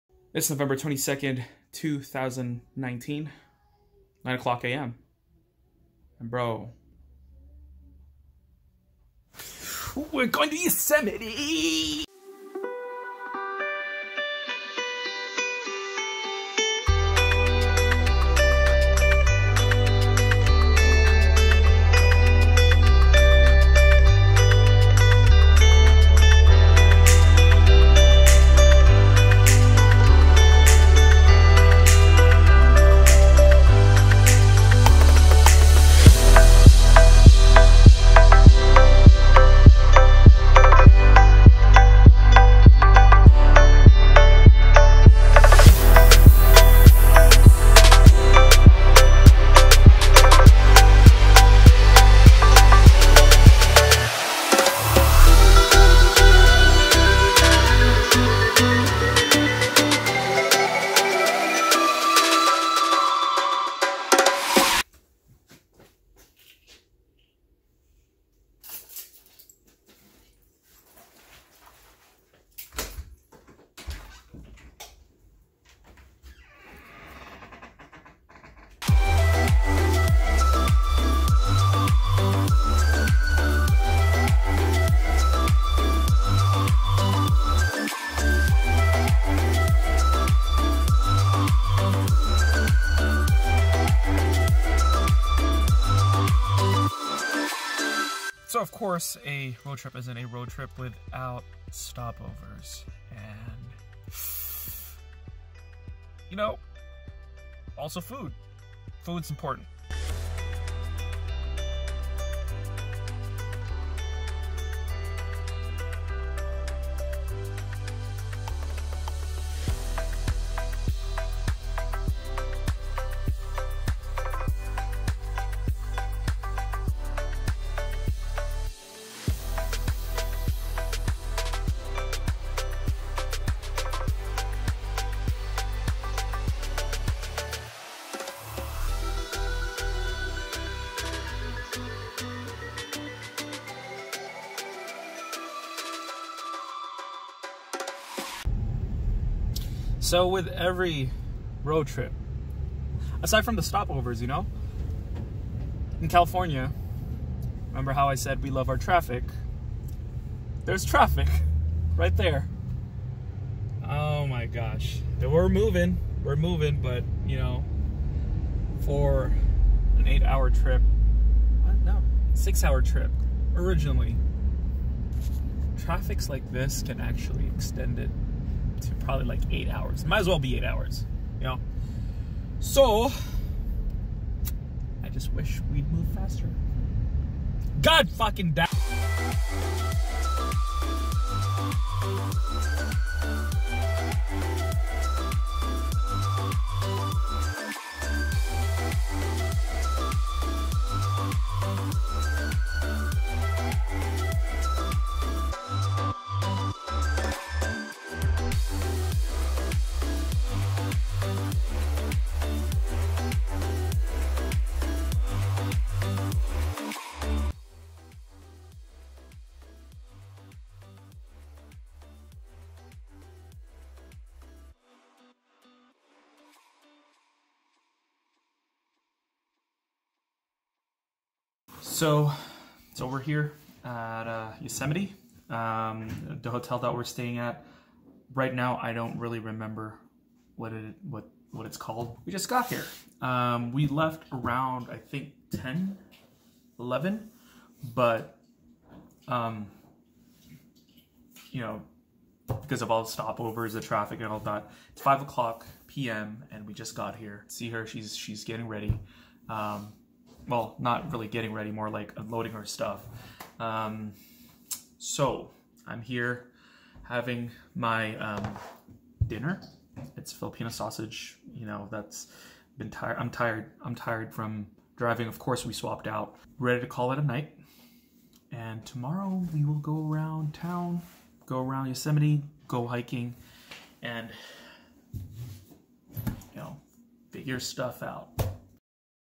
Yosemite Road Trip - THE ROAD TO YOSEMITE!!
For my birthday we decided to go to Yosemite. It's always fun to do road trips, so I decided to do Vlogs whenever I head out. Yosemite is a place that always changes and is unpredictable. It literally is a great place for adventure! Hope you guys like it!

and REMEMBER..

Always Inspire to Adapt!!